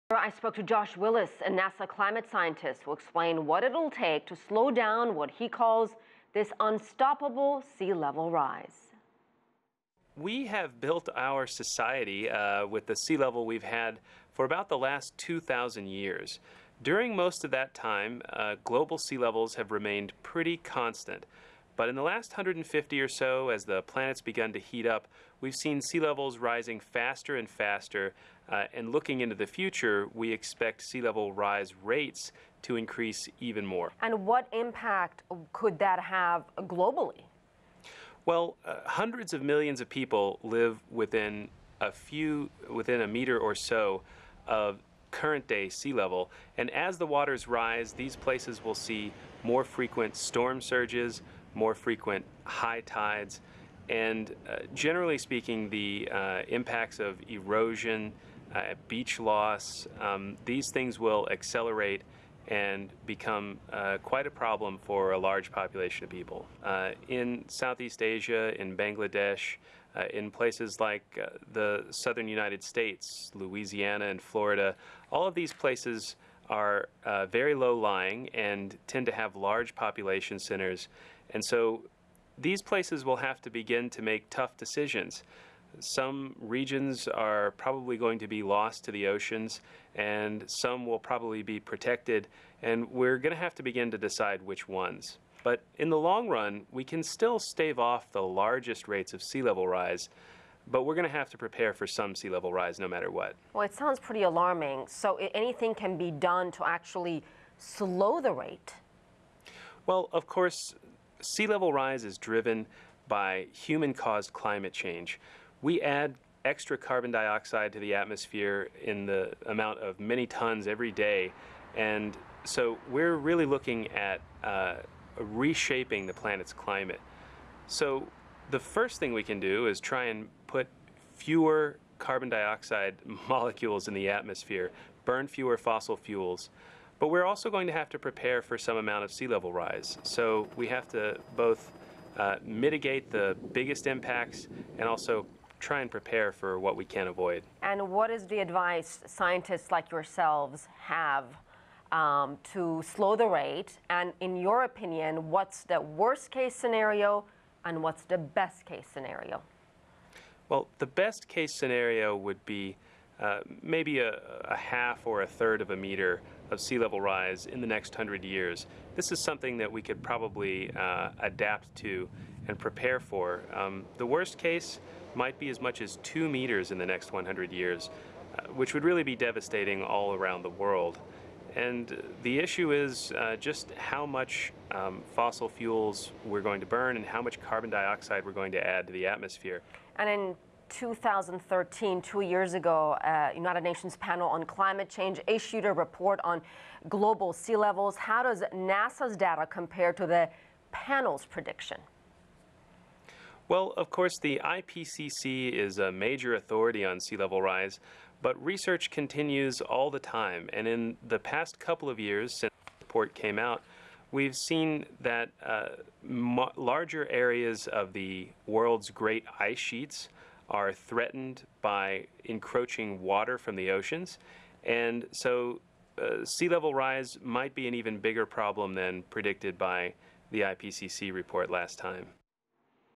Josh Willis on the rising sea levels
CCTV America's Asieh Namdar spoke to Josh Willis, a NASA climate scientist, who explained what it'll take to slow down what he calls "the unstoppable sea level rise".